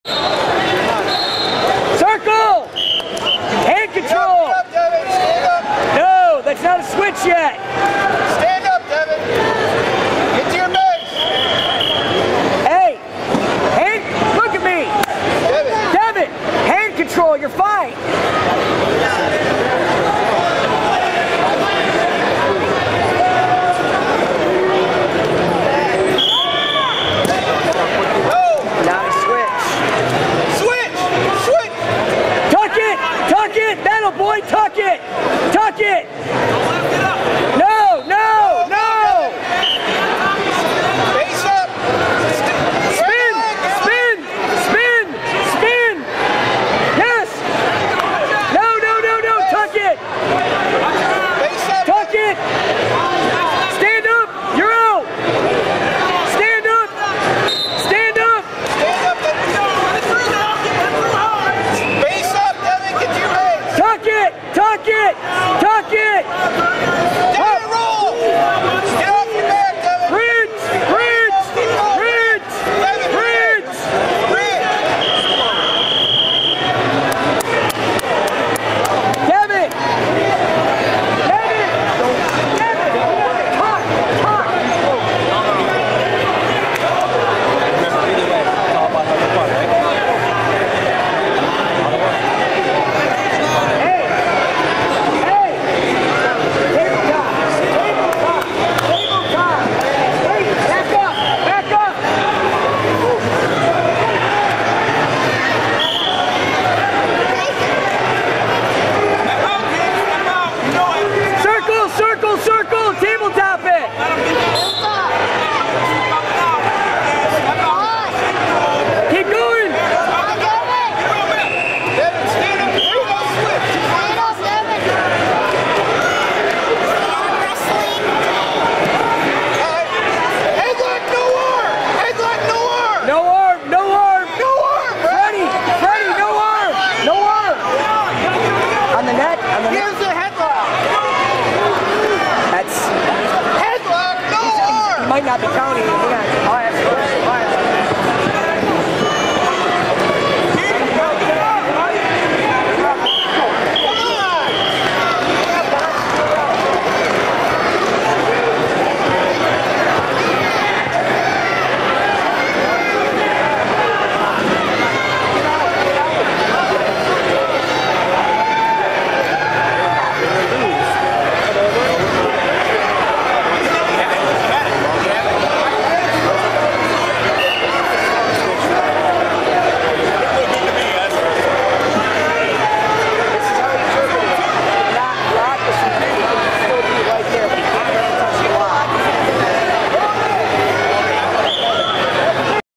Devon Wynacott in round 1 in bout 5.mpg
Grand Ledge Wrestling Open
Sunday, Jan 9, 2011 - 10am on
Grand Ledge HS, Jenne St., Grand Ledge, Michigan - USA
Devon Wyanacott, Grand Ledge Wrestling Club, 50lbs
Round 1, Bout 5